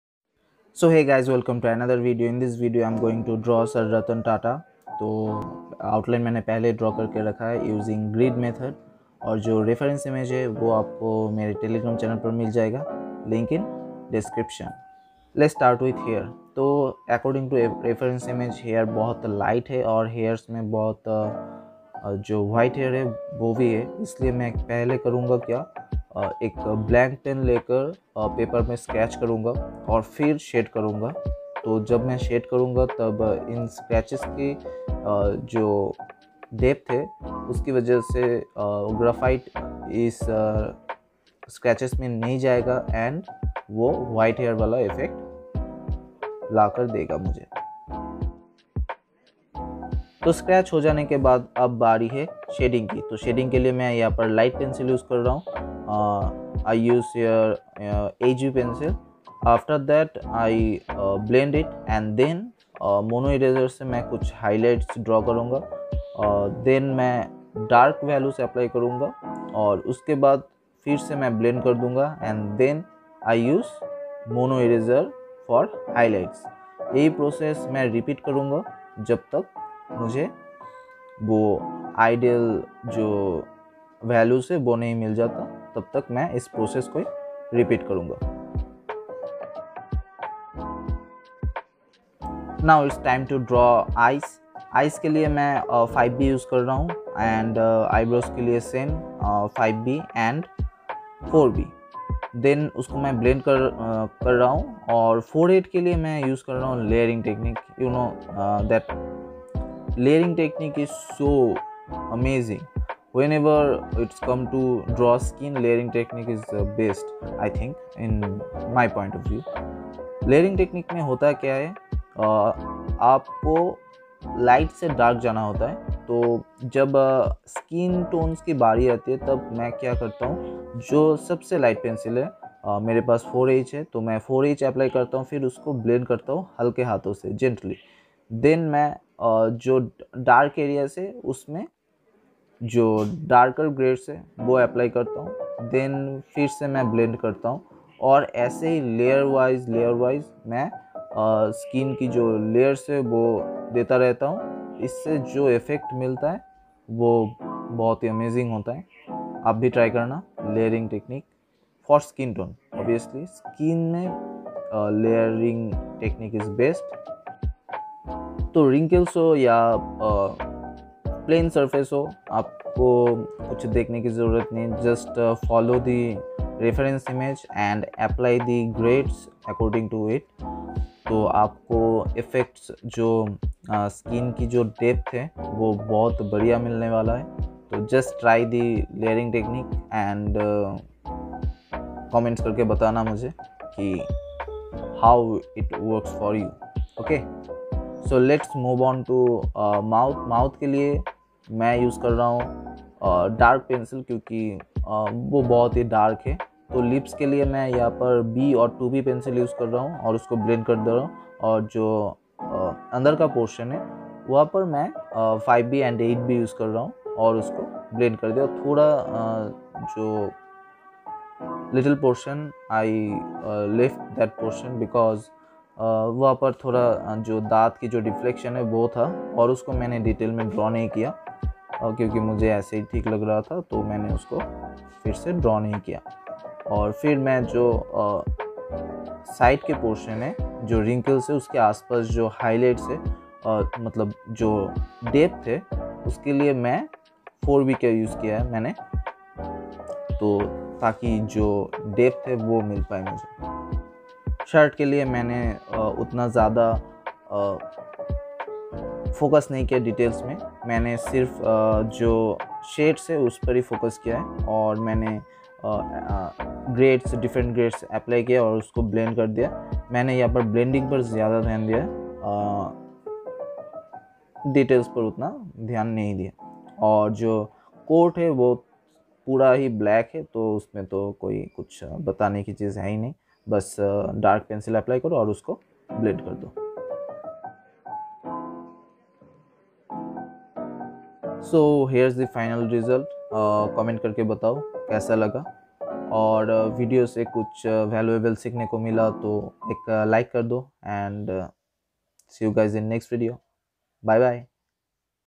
Drawing Sir Ratan Tata - Portrait Tutorial | Sourav's Artwork
Hey Guys, in this video i,m going to draw  Sir, Ratan Tata. We Indians know that the importance of Ratan Tata and What he and His Family did for India. So, here's the a little tribute for Sir Ratan Tata. For this Drawing i use Graphite Pencils and the Paper i use is Ivory 250gsm.

BGM 🎶